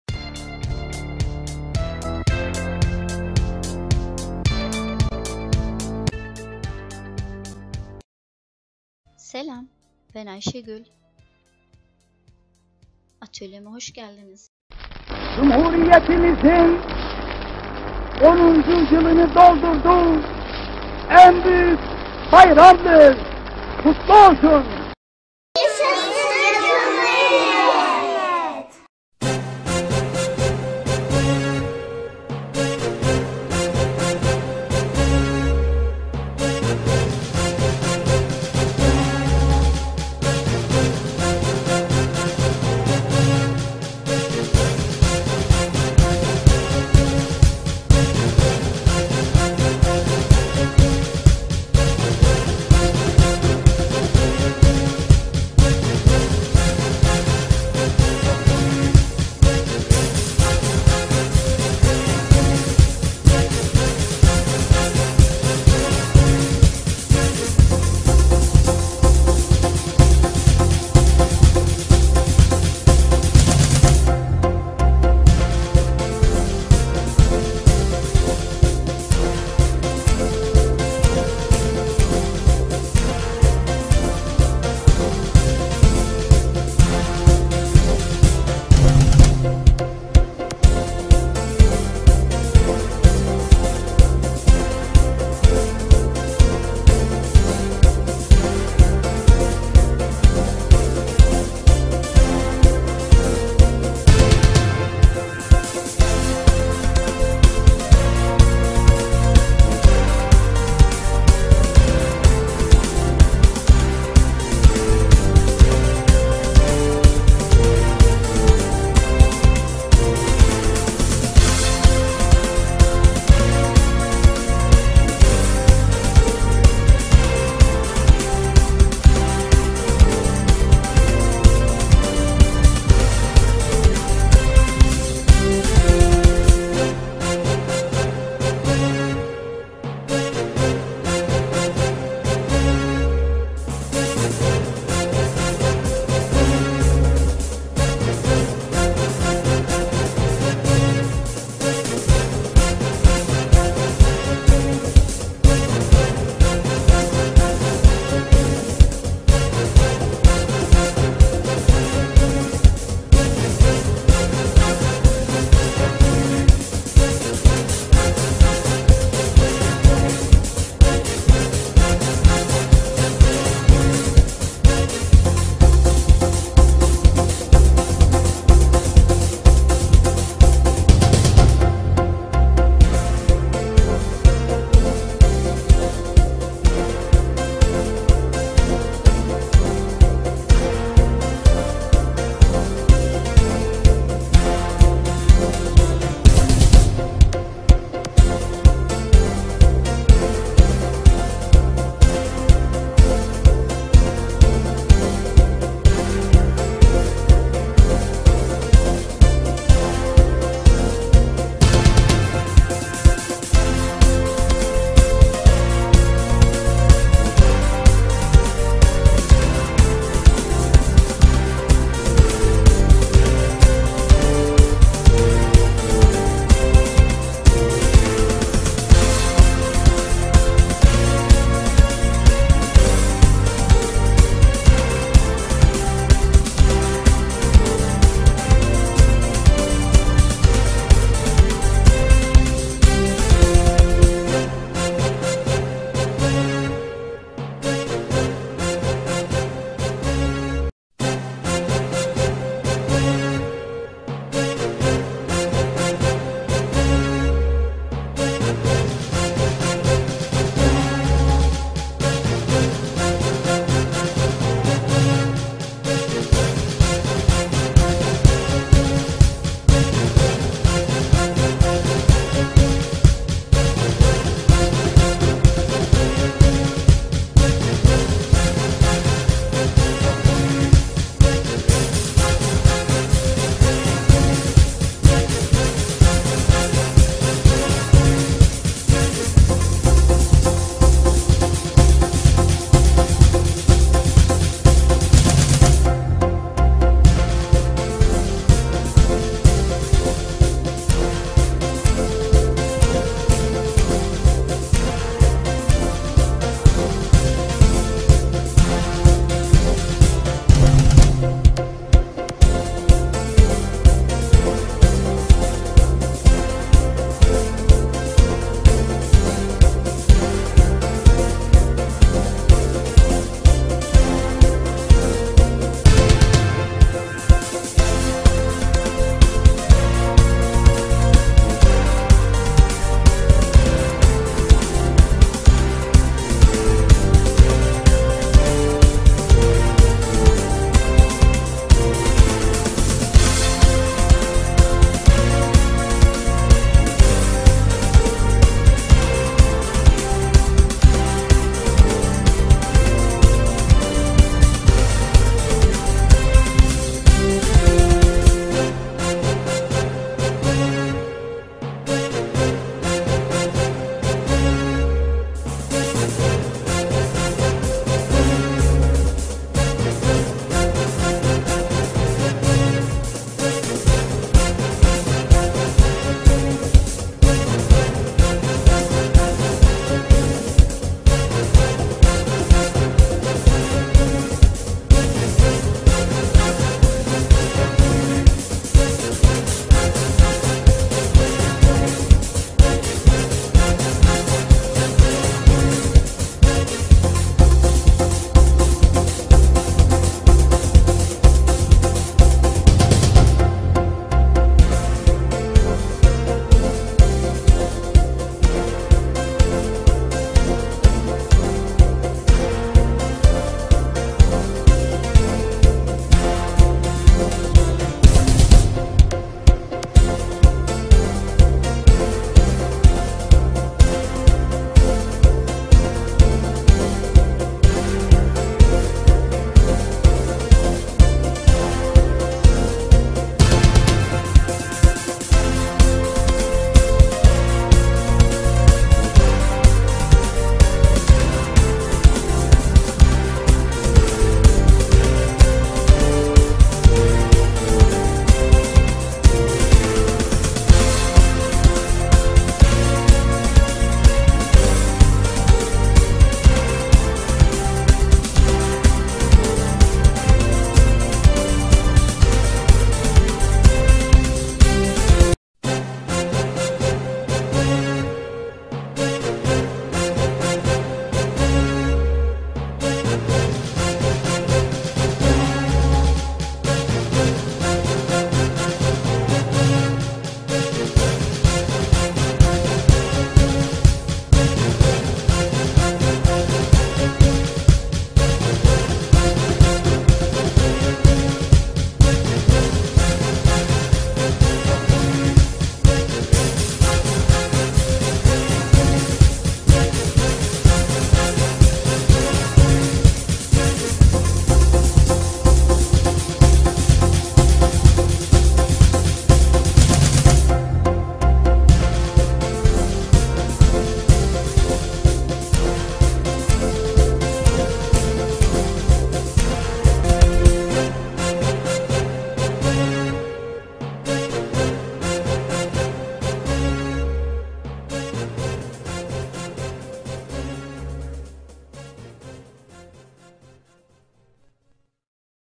DIY | 29 Ekim Cumhuriyet Bayramı Özel Geri Dönüşüm/sınıf süsleme!!!^^
Merhaba. Atölyeme hoş geldiniz 🖐Bugün sizlere, strafor ve eski kıyafetlerden, 29 ekim Cumhuriyet Bayramı için, neler yapılır ;) geri dönüşüm fikri ile geldim. bu videodan sonra kendiniz yapabilirsiniz
 ( you can do it yourself after this video)